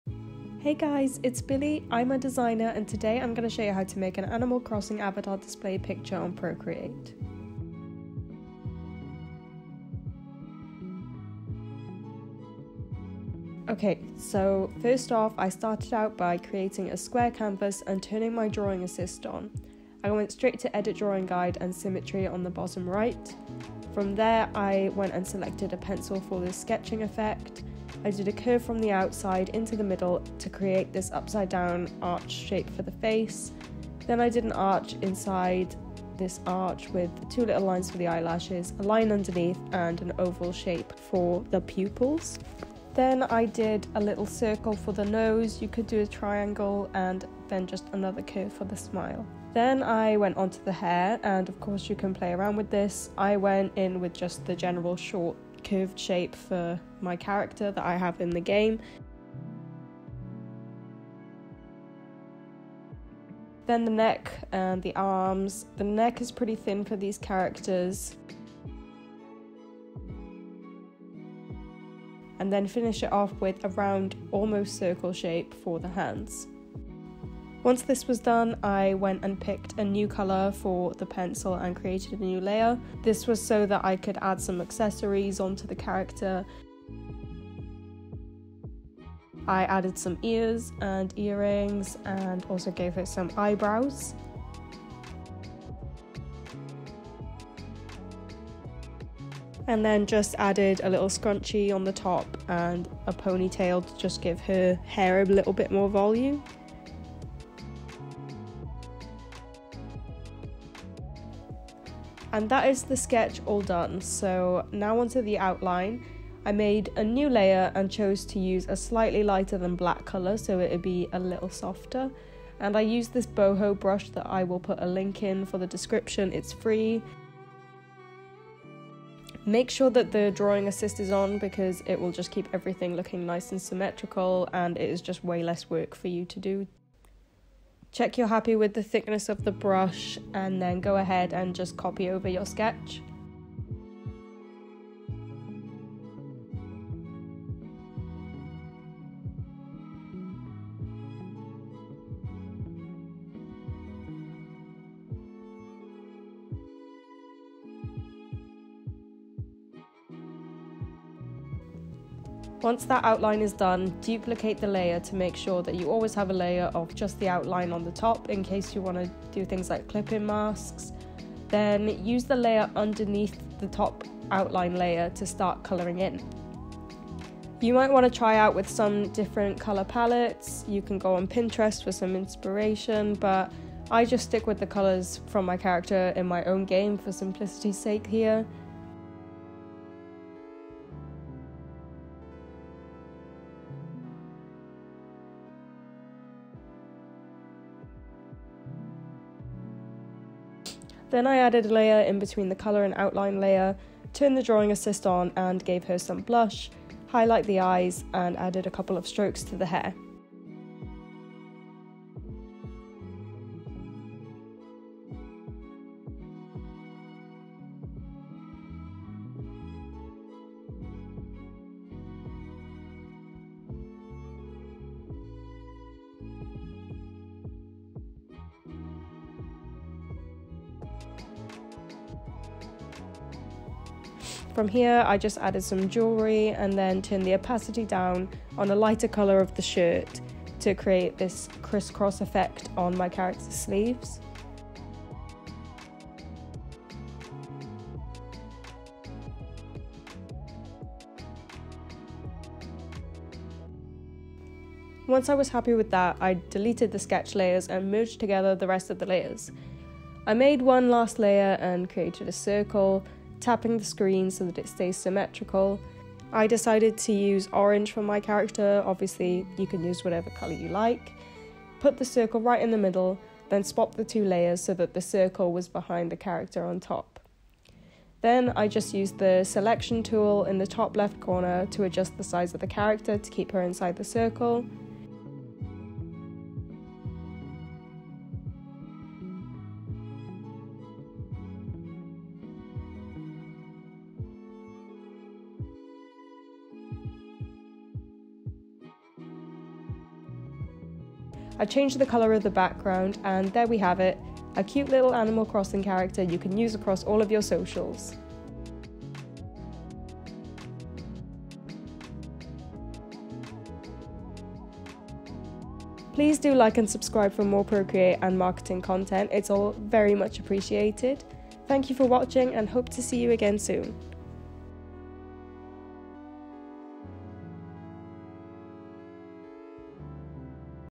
Making an ANIMAL CROSSING AVATAR on PROCREATE - YOU can draw this!┃Procreate tutorial
Are you a fan of Animal Crossing? Or know someone who is?! Well here's how you can make your own display picture of an Animal Crossing character using Procreate to use across your social channels!

Boho Brush Set Download (Free!)